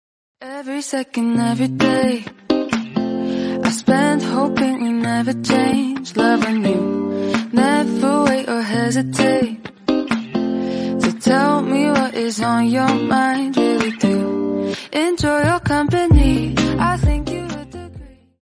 Dissolution du papier toilette confort Popee
Si certains d'entre vous se demandent si notre papier toilette confort se dissout correctement, la réponse est oui !
Et en plus, il est compatible avec tout types de WC 🚽